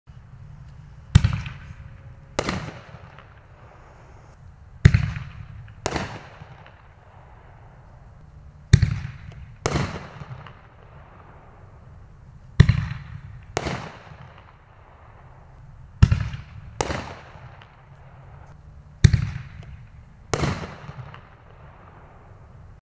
RL101 Black Box Artillery - 1.75" Shells
Classic black box individual artillery shells. This box includes multi-colored shells that everyone will enjoy!